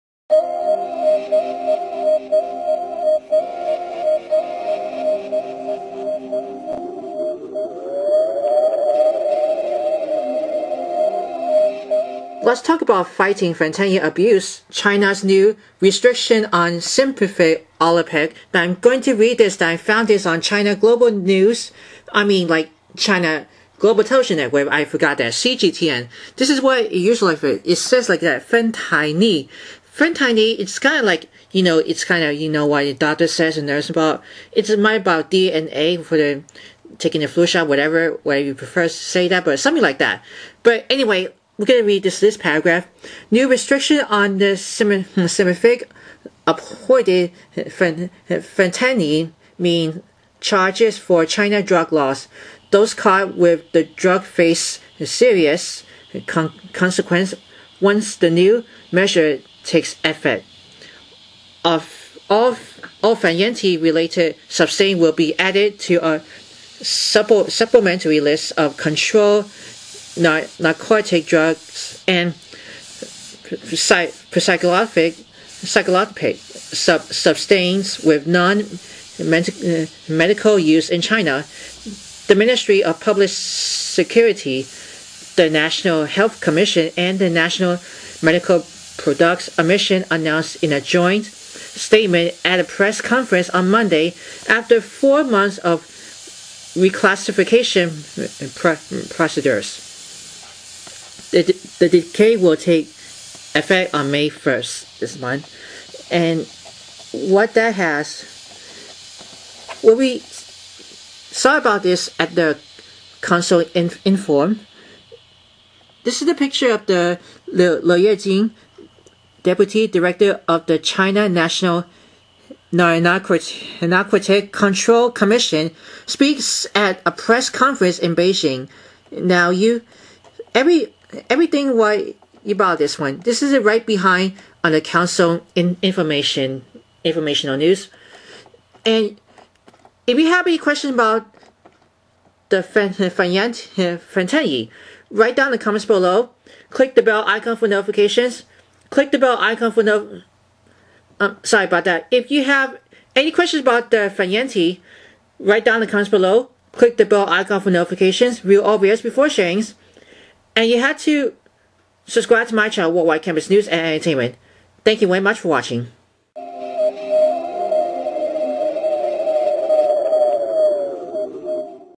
What’s on World Wide Campus? - China’s move against dangerous opioid fentanyl
This weekly roundup for “What’s on World Wide Campus?”, here is the latest news about Fighting fentanyl abuse: China’s new restrictions on synthetic opioid.

According to CGTN, New restrictions on the synthetic opioid fentanyl mean changes for China's drug laws. Those caught with the drug face serious consequences once the new measure takes effect.‍

All fentanyl-related substances will be added to a supplementary list of controlled narcotic drugs and psychotropic substances with non-medical use in China, the Ministry of Public Security, the National Health Commission and the National Medical Products Administration announced in a joint statement at a press conference on Monday, after four months of reclassification procedures.

The decision will take effect on May 1.